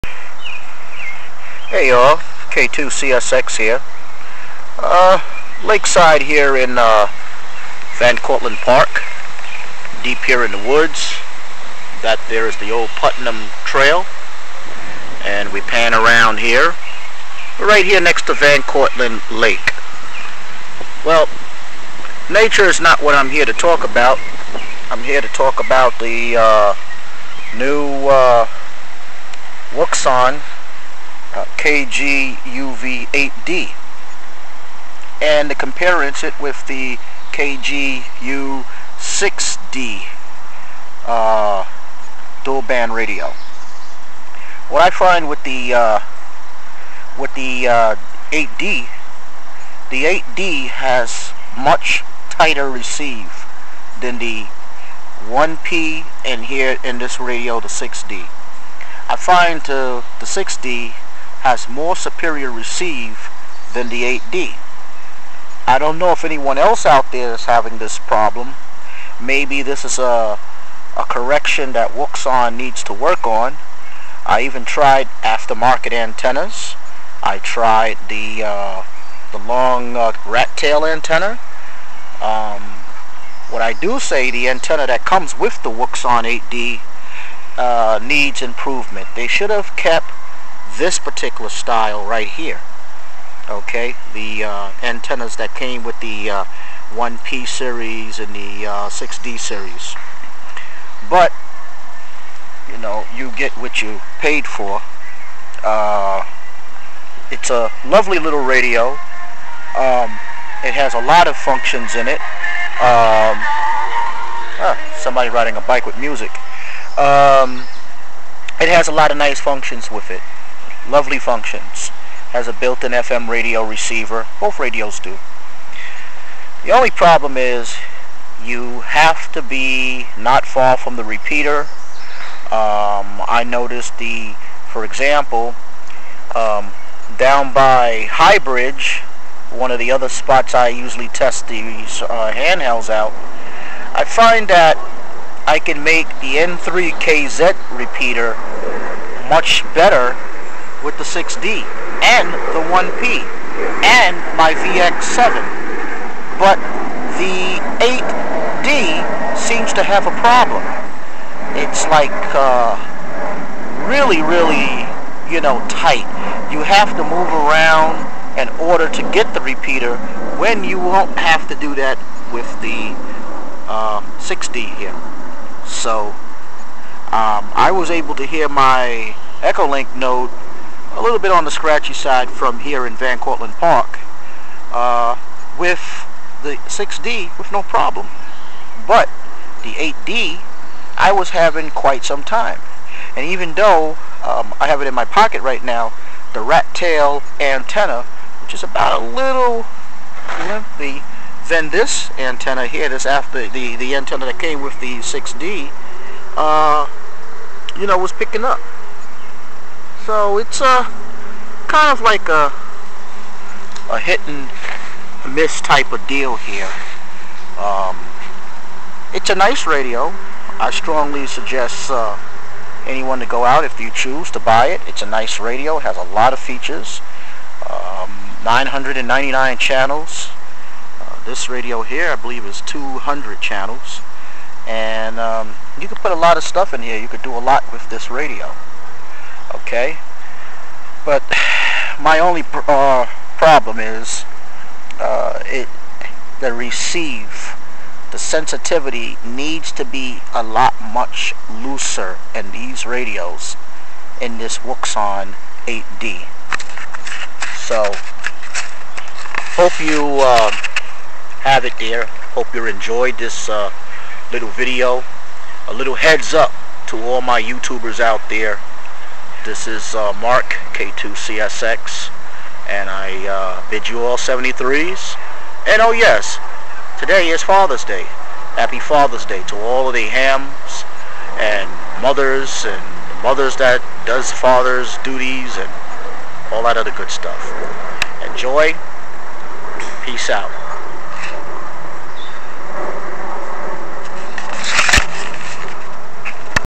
k2csx a updated look at the wouxun kg-uvd-8 ht.
my video will talk about the receiver.i fiend this ht to be a hit ultimately the capability of cross band repeat 999-ch are great.if you take a 8d and test the receive next to the 1p or 6d the'y are not the same. but all and all it's a great little radio.73's mark k2csx